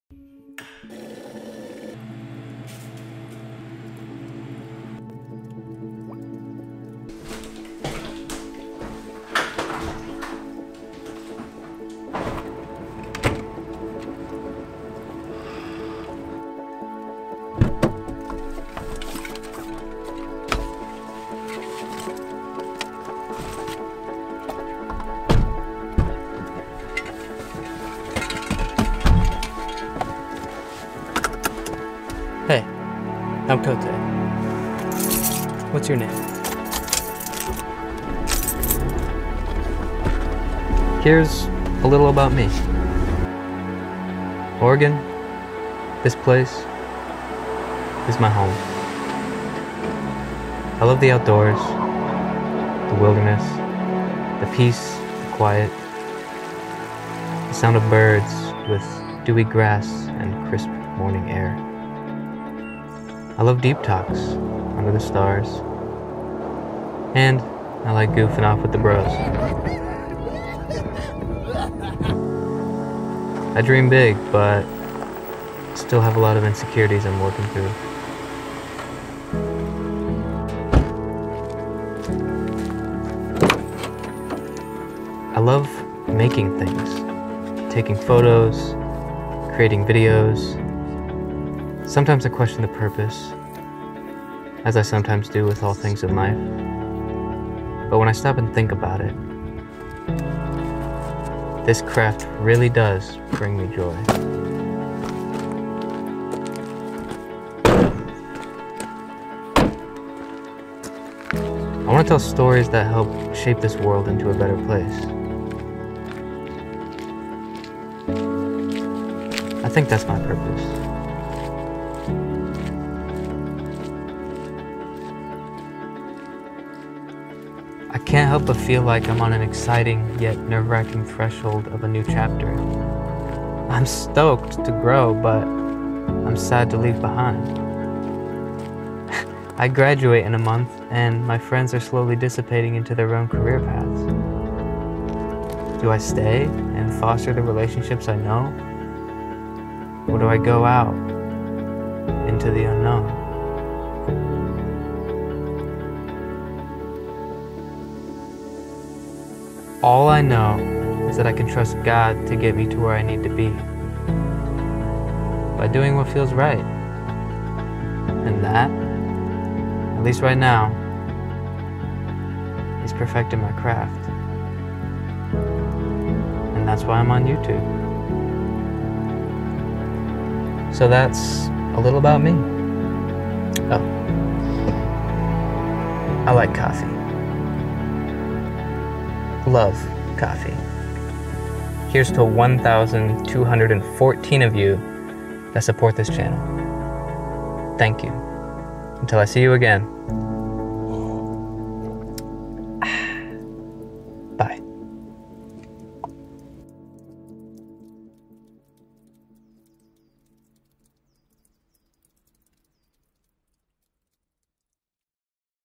a little about me
Here's a quick introduction video about me and the thoughts that run through my head. If y'all wanna know more about me, I can make a more traditional style intro video. I just figured I'd get a little creative with cutting the small talk stuff. Welcome if you're new!

Be sure to check out last weeks video!

Sony a6500
Rode VideoMicro
Zoom H1n + Lav
Mavic Air